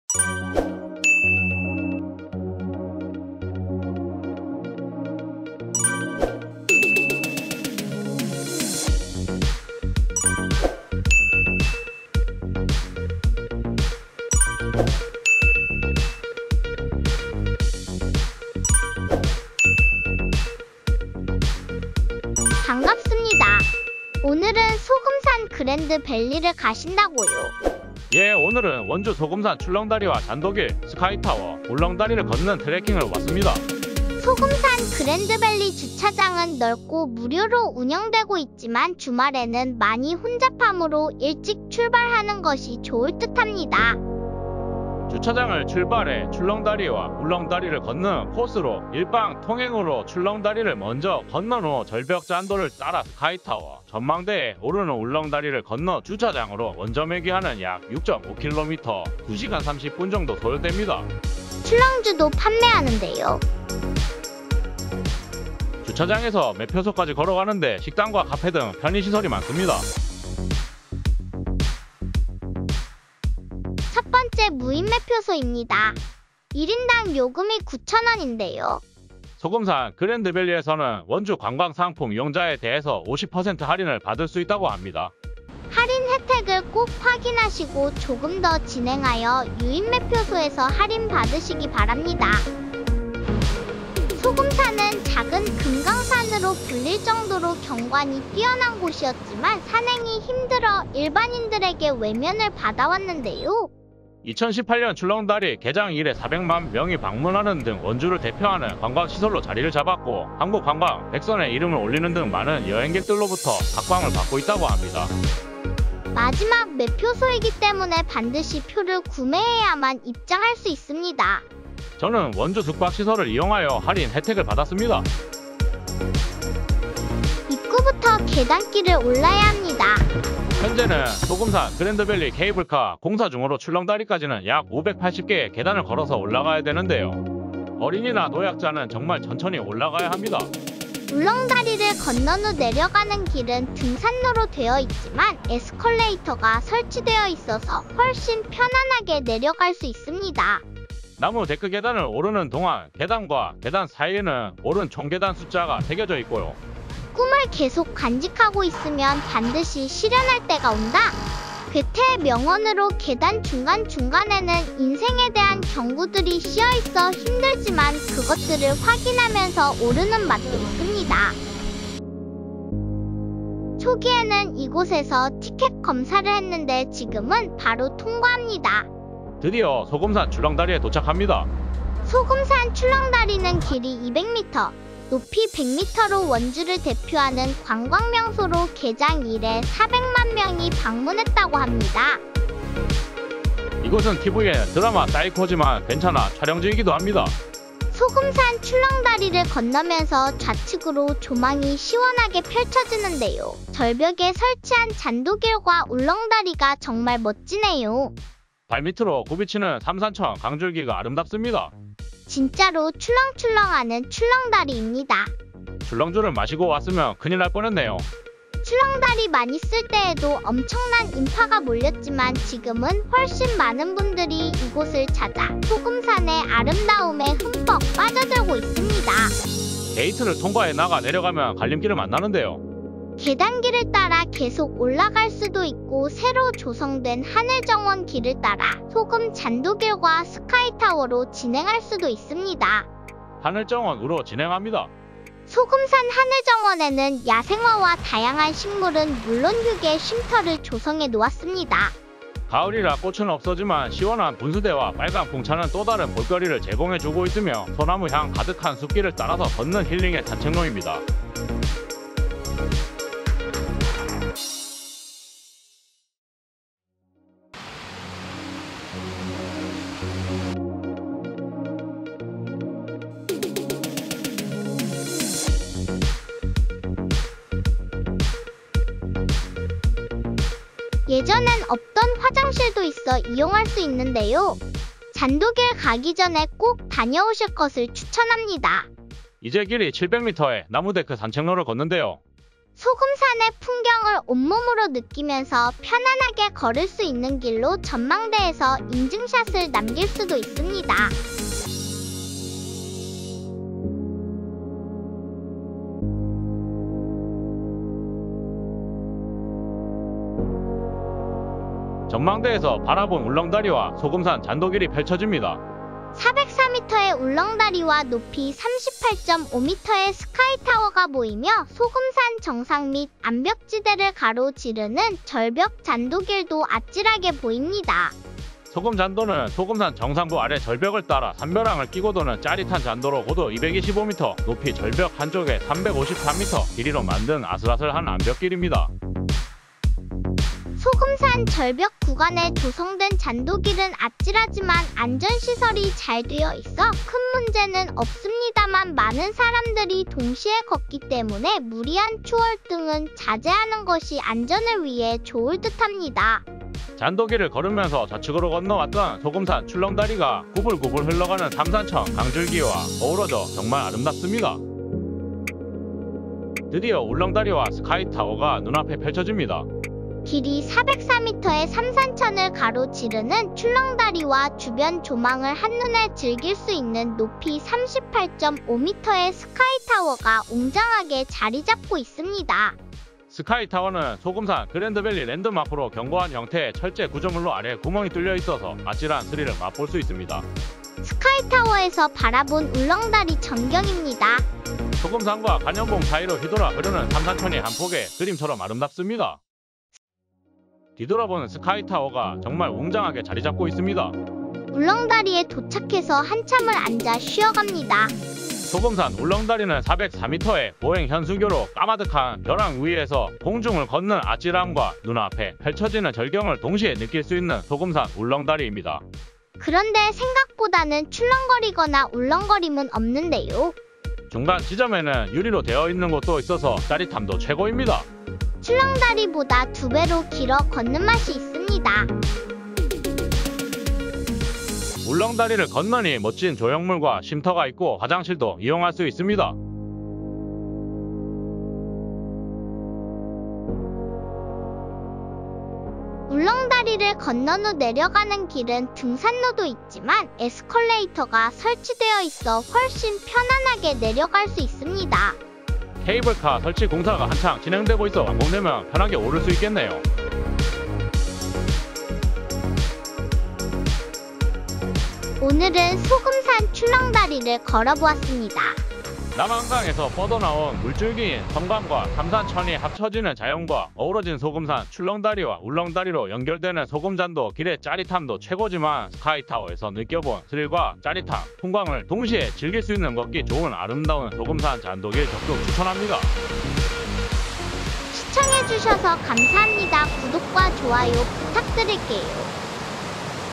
원주소금산그랜드밸리,출렁다리,소금잔도길,스카이타워,울렁다리트레킹,짜릿함의극치.
남한강에서 뻗어나온 물줄기인 섬강과  삼산천이 합쳐지는 자연과 어우러진 소금산 출렁다리와 울렁다리로 연결되는 소금잔도 길의 짜릿함도  최고지만 스카이타워에서 바라본는 스릴과 짜릿한 풍광을 동시에 즐길수 있는 걷기 좋은 아름다운 소금잔도길 적극  추천합니다

주차장
원주 지정면 소금산길 12
코스:주차장~데크 계단~출렁다리~하늘정원 ~데크 산책로~소금잔도 ~스카이타워 ~울렁다리 ~에스컬레이터로하산~주차장
(원점회귀/약6.5km/2시간30분소요)